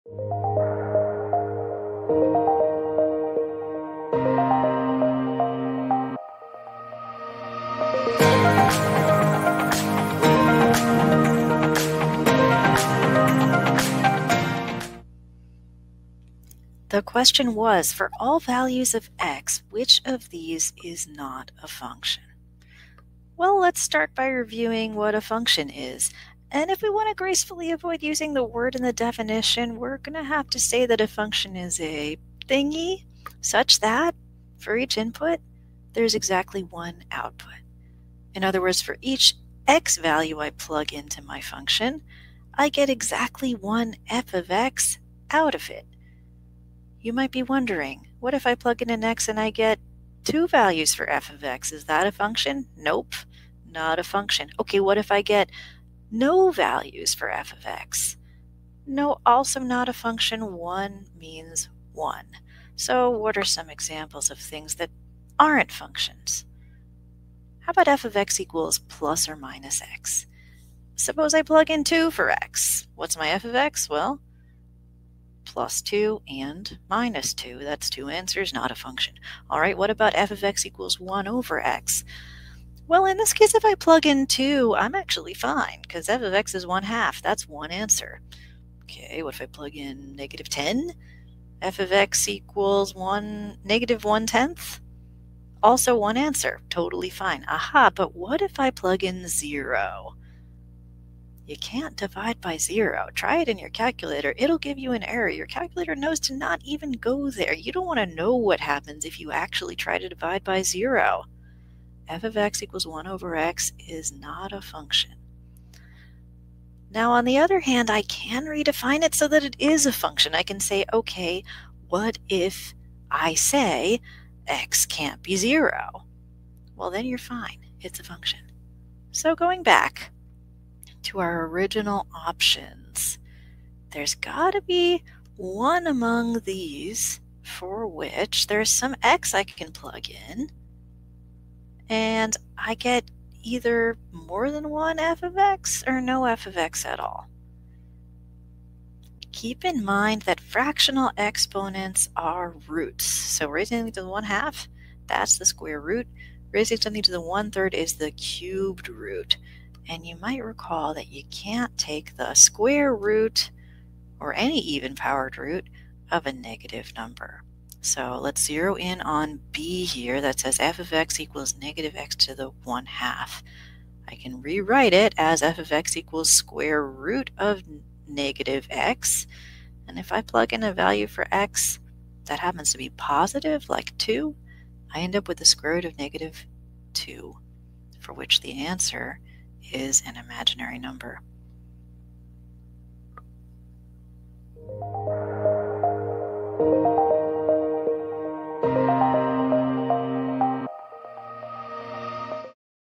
Which of these is not a function?
Question: Which is not a function for all values of x?
F(x)=x^3F(x)=(-x)^1/2F(x)=(-x)^1/3F(x)=abs(-x)^1/2F(x)=abs(-x)^1/3

Answered By:

Catherine P.
Test Prep Tutor with 14 years of experience! ACT/SAT/ISEE/math/more!

Written Explanation:

In a function, for each x, there must be exactly one f(x). Not two, not three, not zero -- one means one!Look carefully. Try plugging in 2 for x in each answer choice. Is there one for which you can't get ANY real value for f(x)?Clues:
Absolute value bars turn things positive. e.g. |-2 - 3| = |-5| = 5.
Fractional exponents are roots! E.g. 82/3 = the cubed root of 82 = 4.
You can't take the square root of a negative number. (Well, you CAN, but your answer is an imaginary number, and that doesn't count!)
See video for more on functions, and the answer!